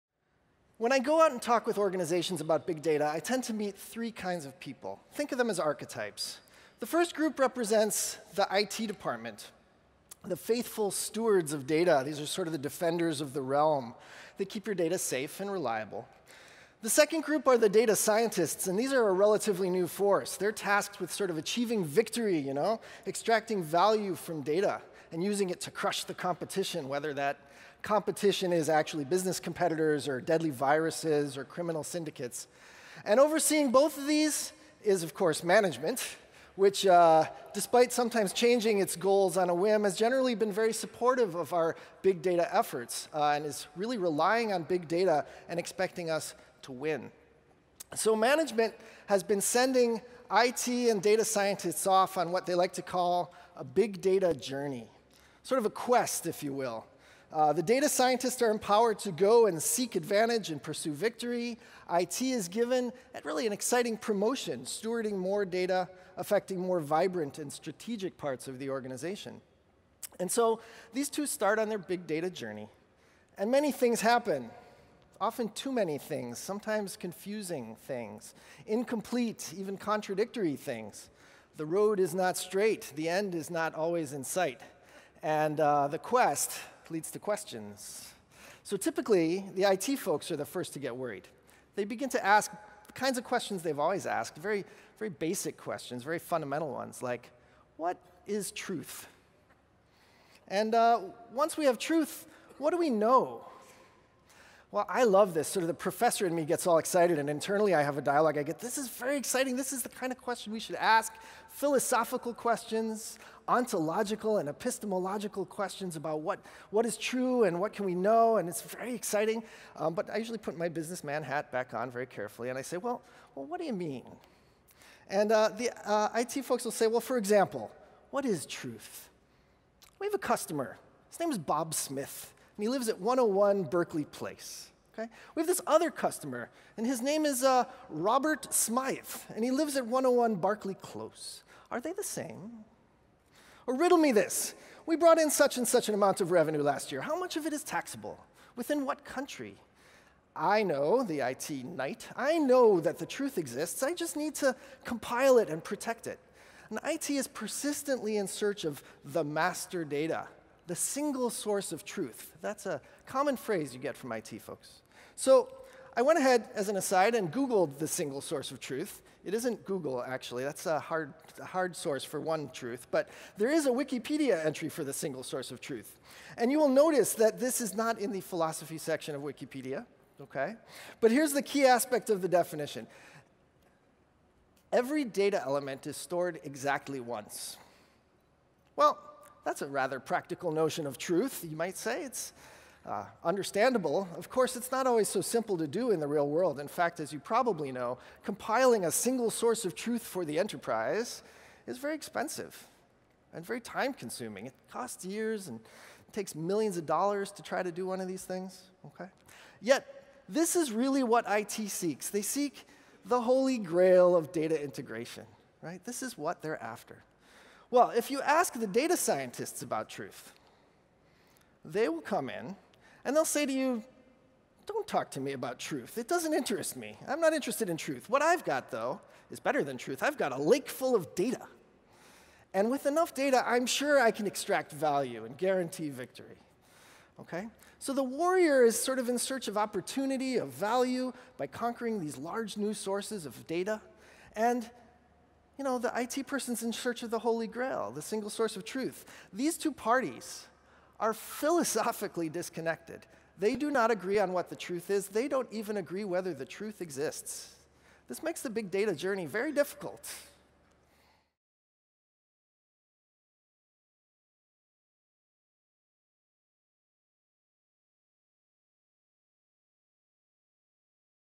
Data Relativism - Joe Hellerstein - Strata + Hadoop World 2016 London
UC Berkeley's Joe Hellerstein explains why we now take a relativistic view data, where the meaning of data depends on the context in which it is used.